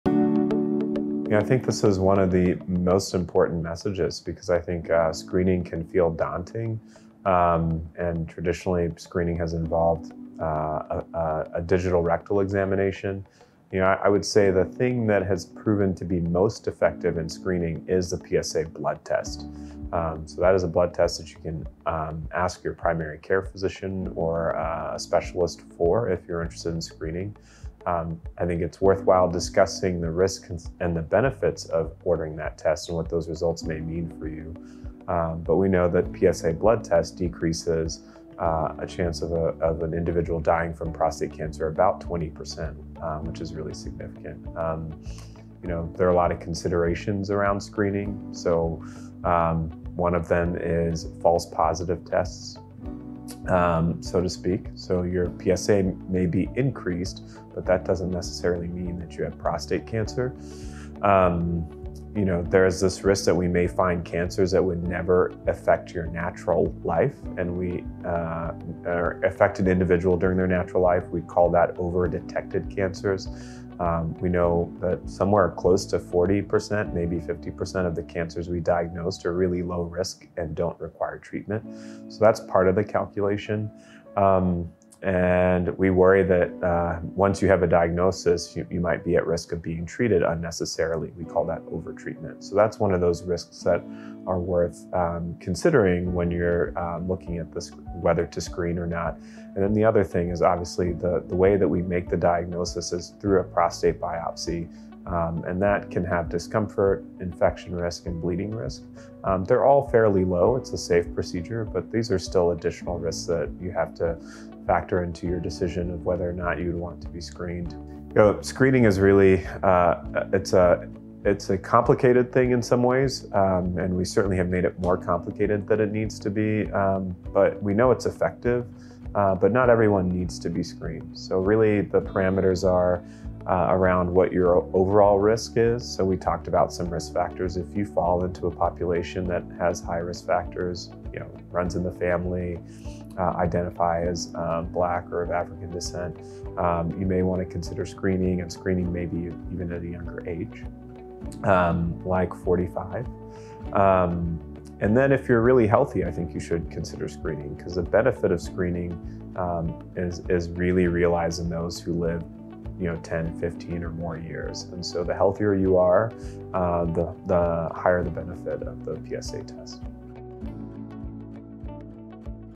Should YOU get screened for prostate cancer?
We know that a Prostate Specific Antigen blood test, or PSA test, reduces the chances of an individual dying from prostate cancer by about 20%, says Fred Hutch urologic oncologist Dr. Yaw Nyame. In this video, he explains why you should talk to your health care provider about the benefits and potential risks of getting screened.